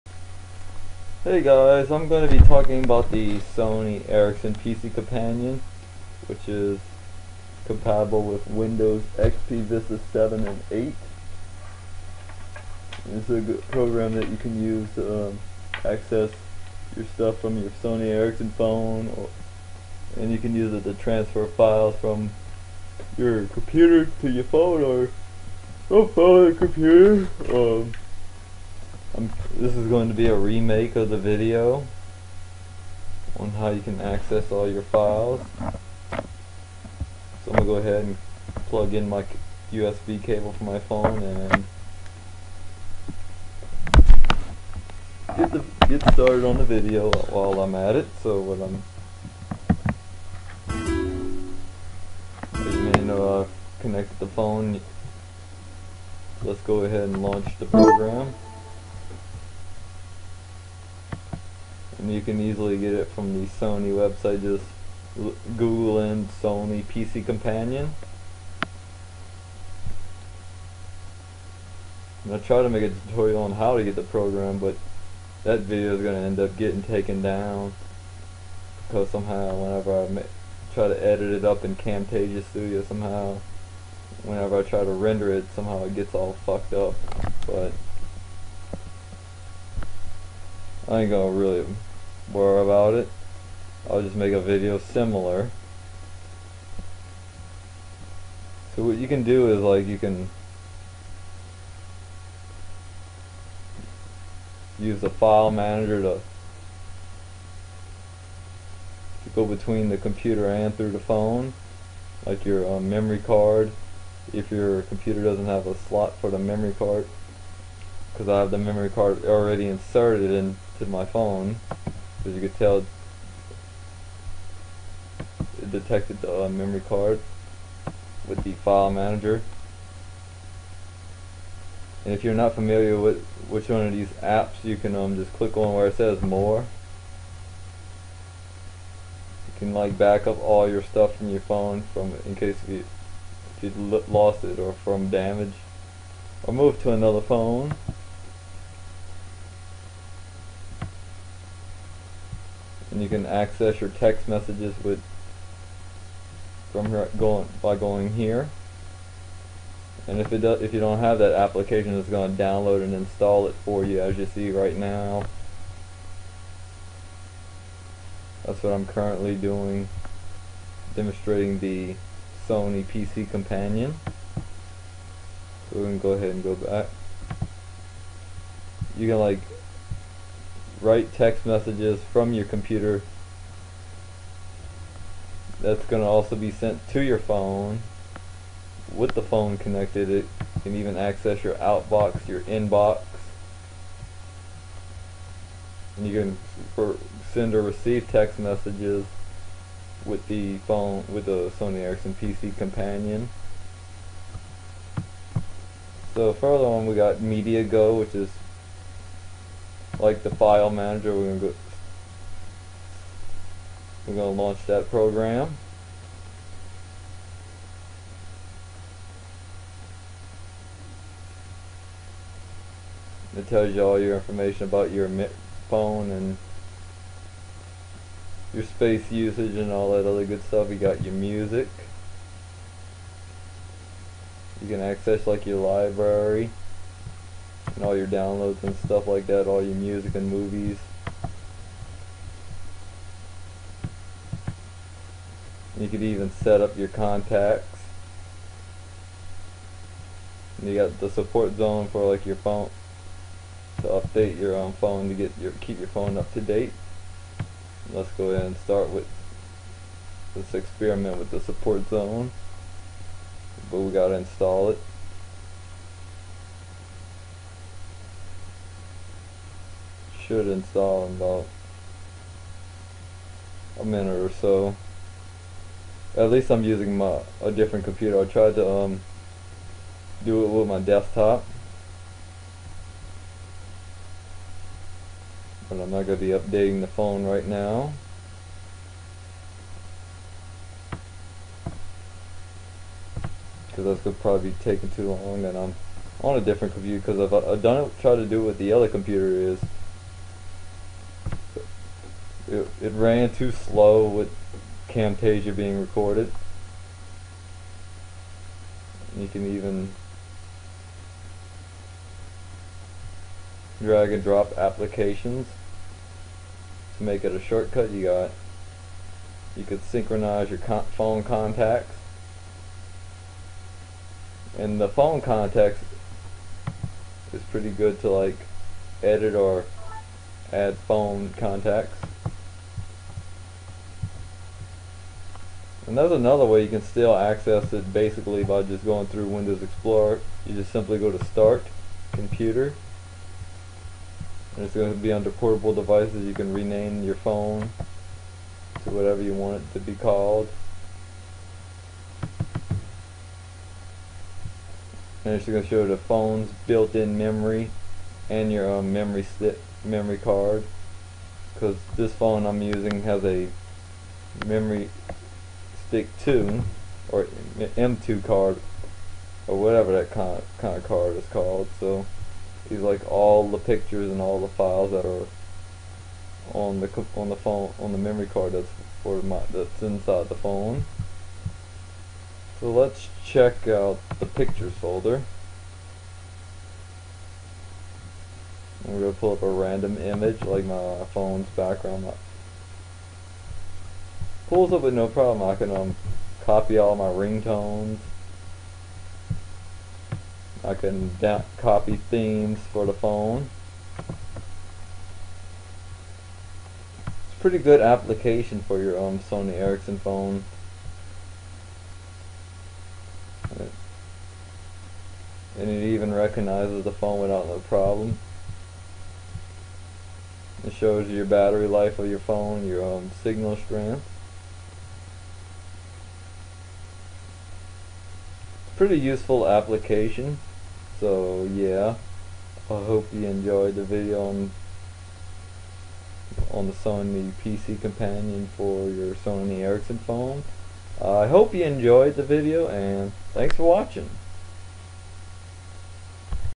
Managing Files On Your Sony Ericsson Phone With The Sony PC Companion
So guys, I'm demonstrating on how you can manage files between Sony PC Companion and Windows Explorer using Sony Ericsson phone on your PC. It's easy to use and I had made a video to get the program somehow the other I took it down I don't know why or how it got part of it half way fucked up so I re-making similar to what I had earlier I wasn't my fault that the last video got fucked so blame it on Camtasia Studio which I was on my desktop in the kitchen. Anyways you'll need Sony Ericsson and USB cable for your Sony Ericsson phone and the PC Companion should detect the drivers.